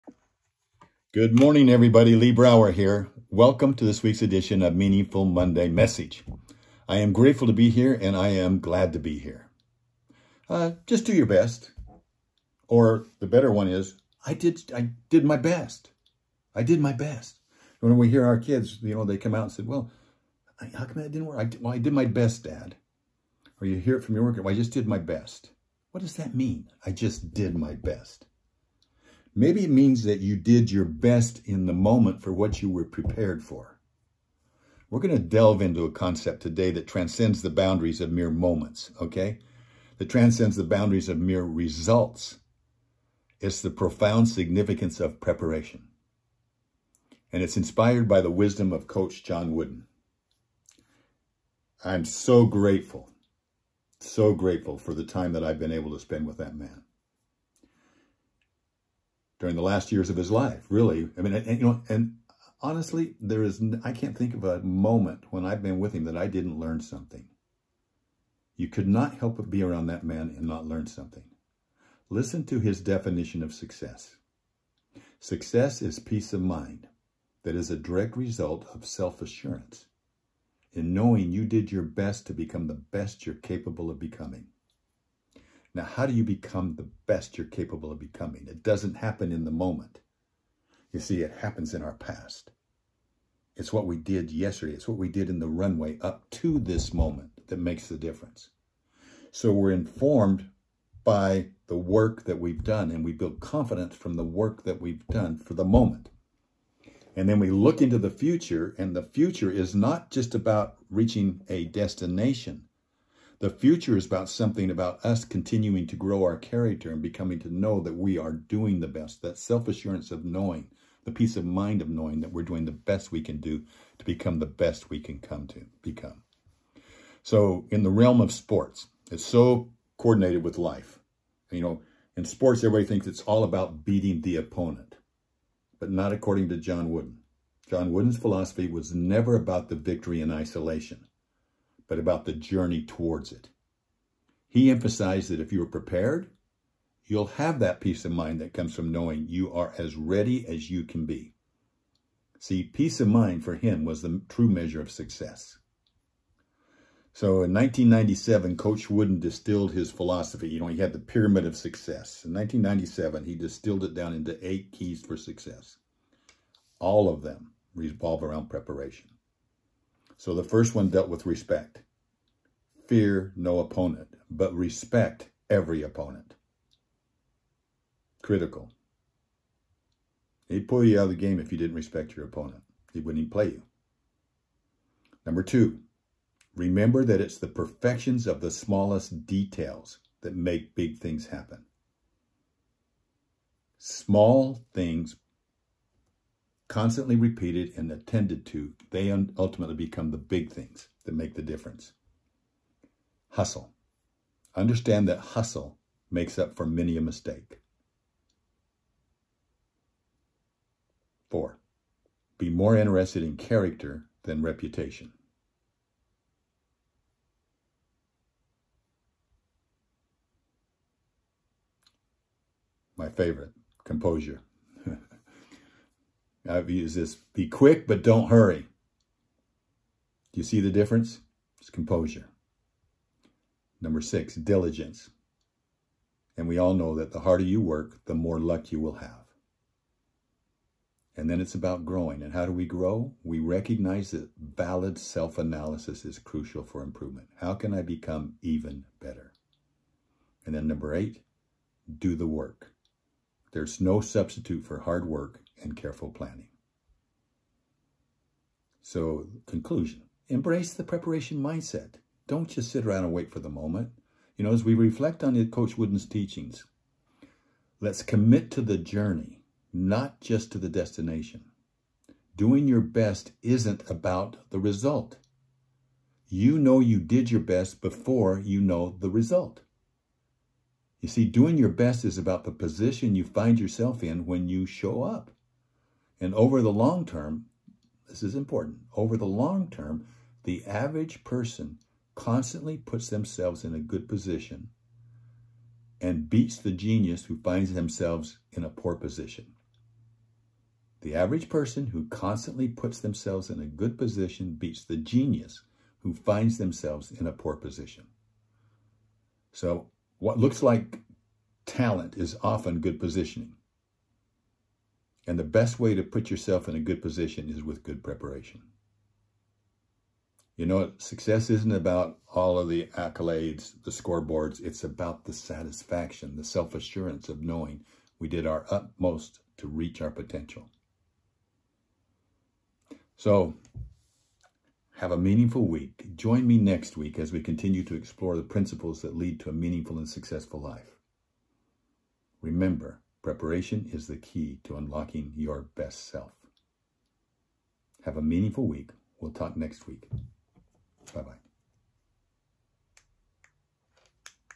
I did my best! Really? A lesson from Coach John Wooden
Doing your best is not about the result. You know you did your best before you know the result.
Over the long term, the average person who constantly puts themselves in a good position beats the genius who finds themselves in a poor position.
Wooden's Eight Rules for Success:
1. Fear no opponent- but respect every opponent.
2. Remember that the perfections of the smallest details make big things happen.
3. Understand that Hustle makes up for many a mistake.
4. Be more interested in Character than reputation.
5. Be quick, but don't hurry.
6. Know that the harder you work, the more luck you will have.
7. Recognize that valid self-analysis is crucial for improvement.
8. There is no substitute for hard work and careful planning.